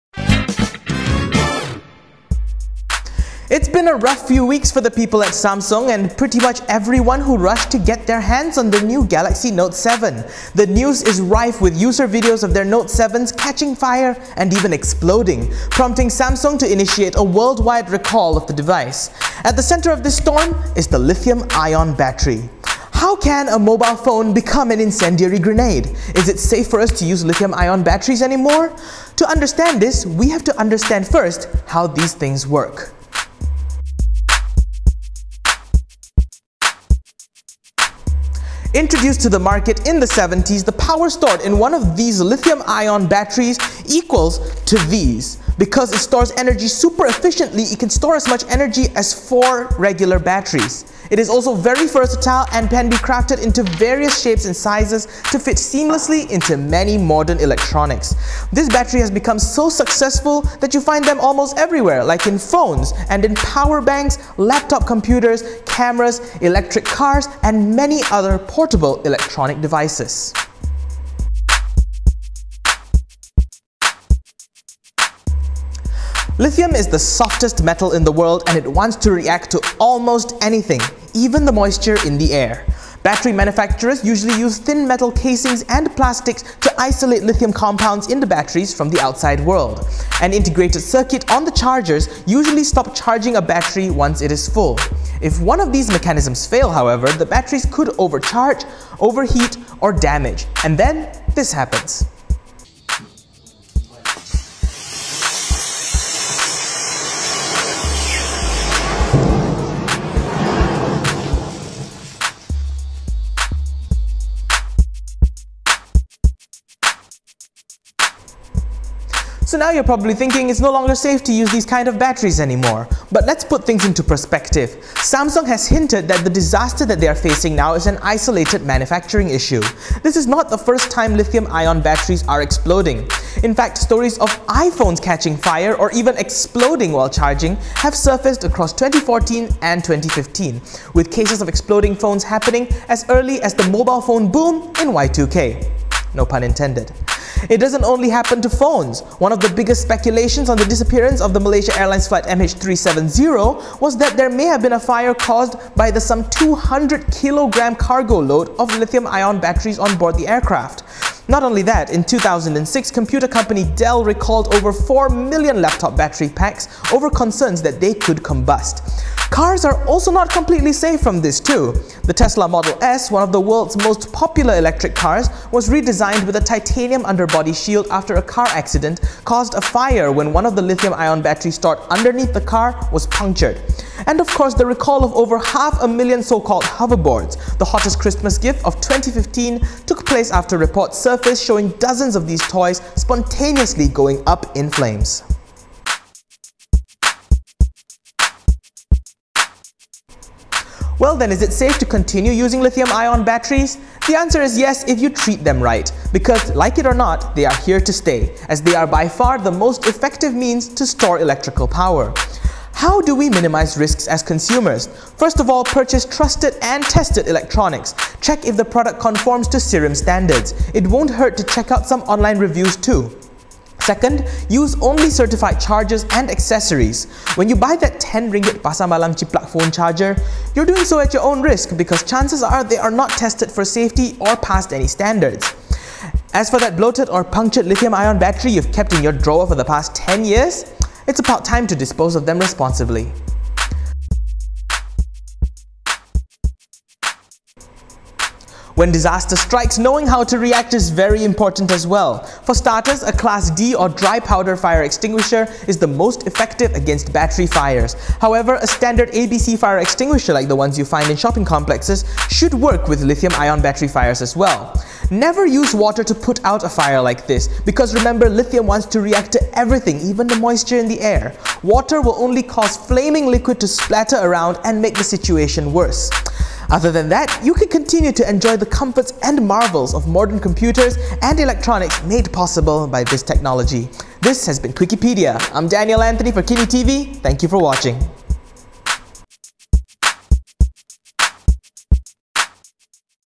Quickiepedia: Why do mobile phones explode?
Could a piece of technology almost 40 years old be what's responsible for turning cellphones into claymores?